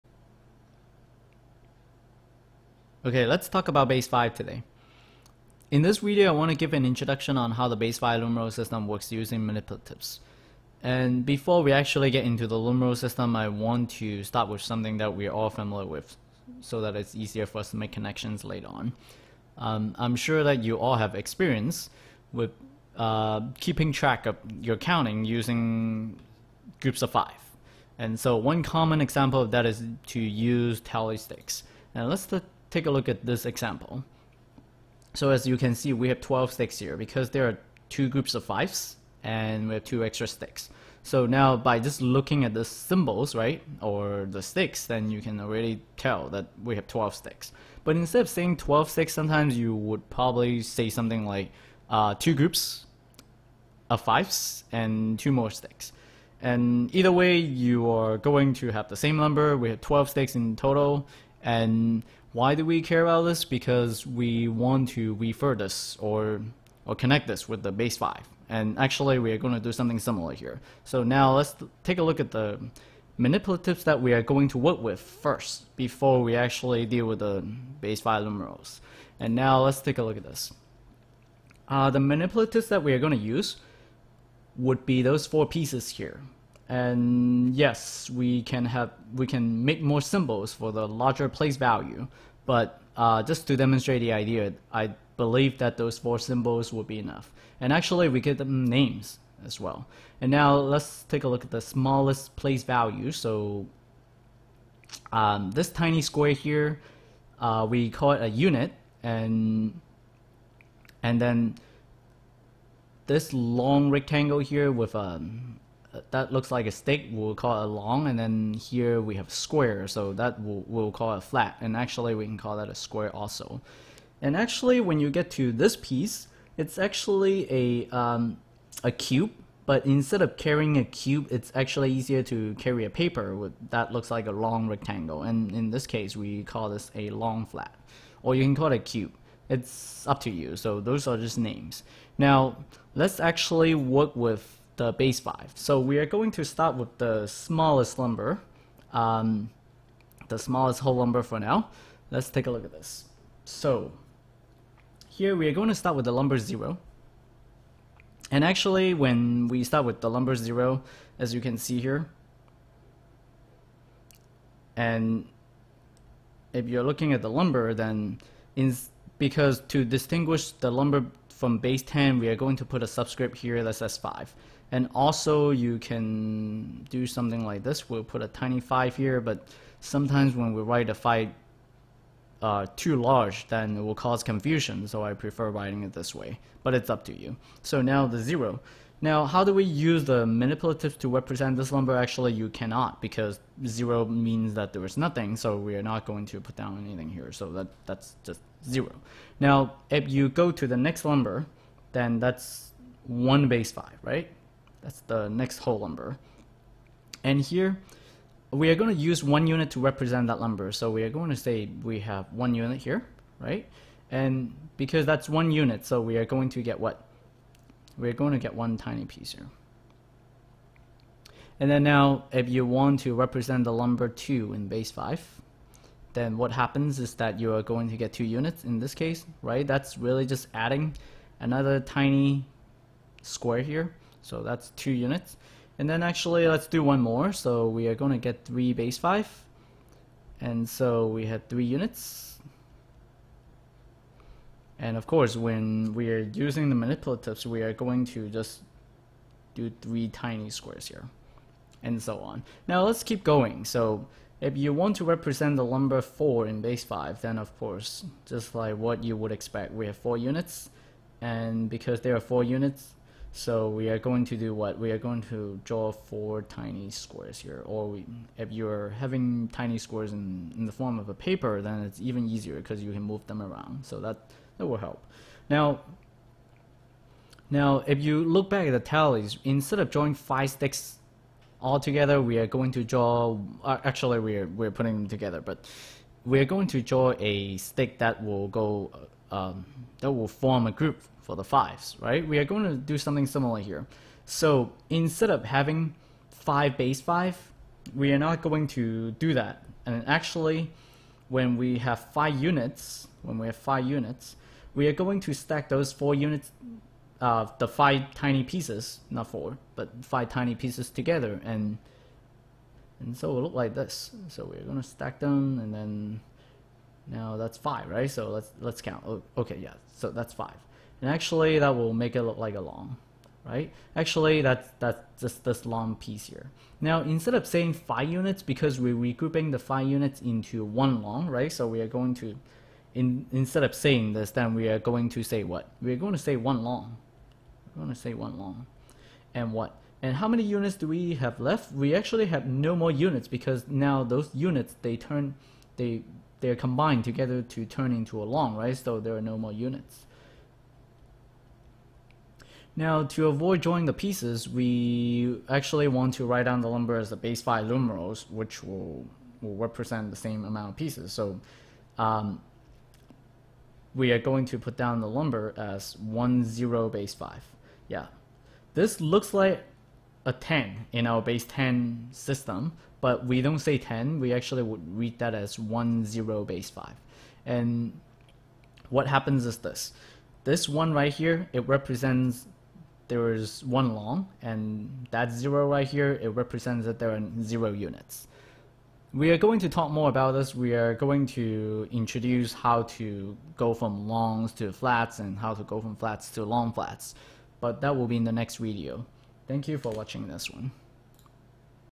Base Five 01 - Introduction and Manipulatives Part 1
We look at the base five and see how it relates to our base 10 number system. (They behave the same way!)

I will show in other videos how to perform addition, subtraction, multiplication, and division in base five, and see how we do such calculations the same way in base 10.